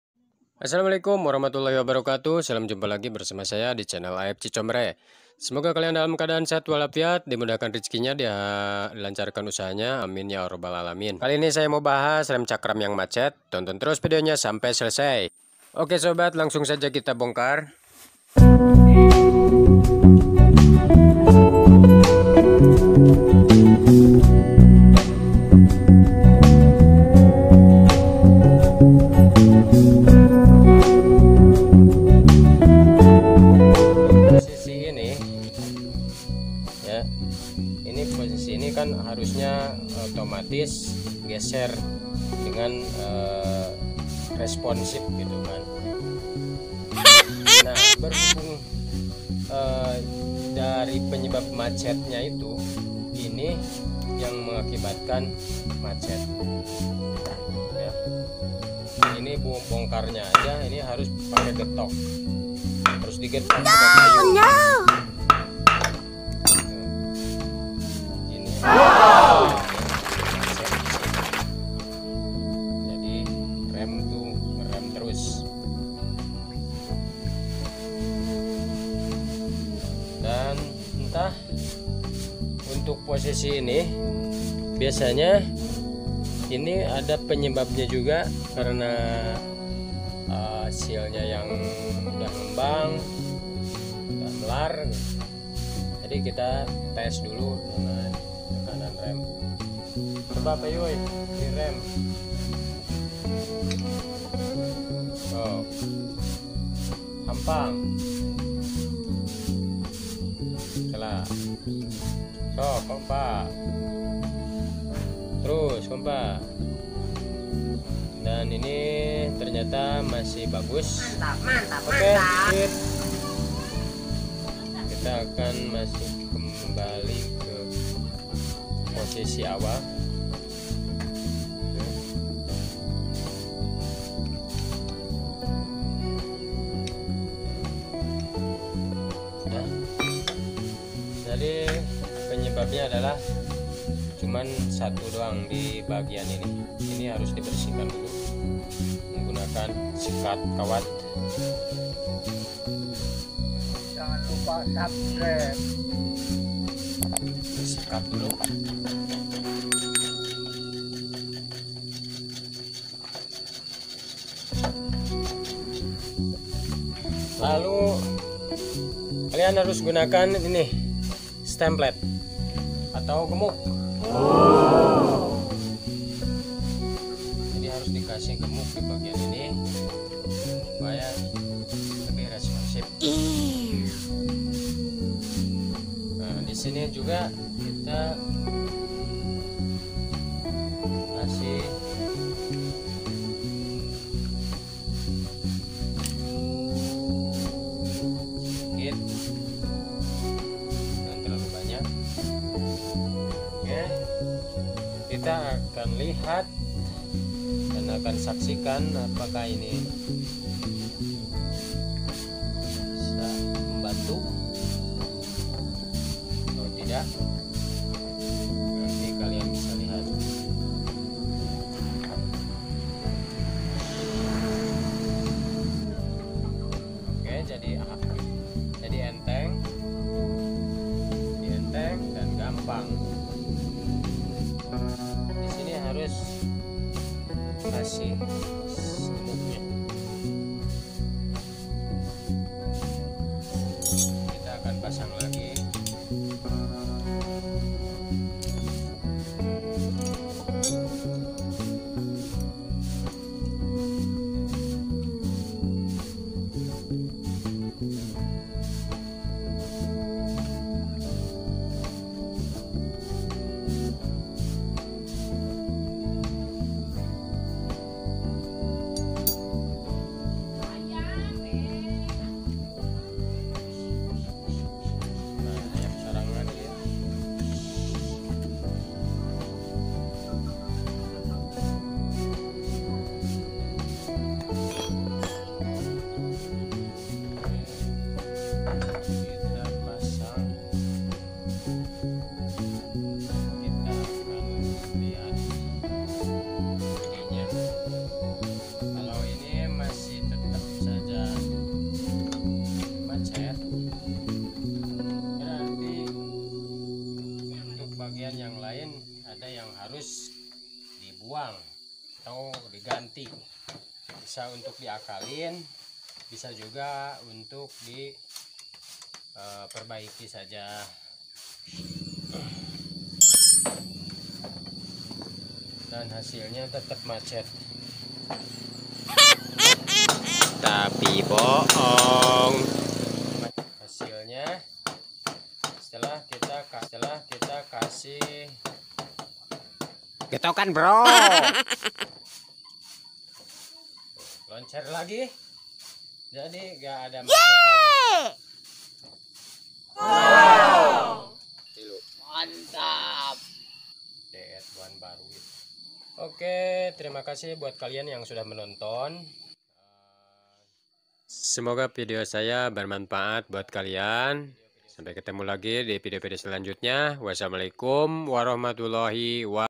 Solusi Perbaiki Rem Cakram Macet Pada Motor!!!
di video kali ini akan membahas tentang Perbaiki Rem Cakram Macet Pada Motor!!! terimakasih...

cakram yang macet akan berakibat fatal jika tidak diperbaiki hal ini sangat penting dan harus selalu diperhatikan dan ada 2 hal yg menjadi penyebabnya antara dibagian sil piston cakram dan as stabil cakram,disini hanya satu hal yang menjadi penyebabnya yaitu bagian as stabil cakram yg dimana tidak responsip yang diakibatkan oleh penumpukan karat sehingga jika saat di rem terjadi macet gak balik ke semula...terimakasih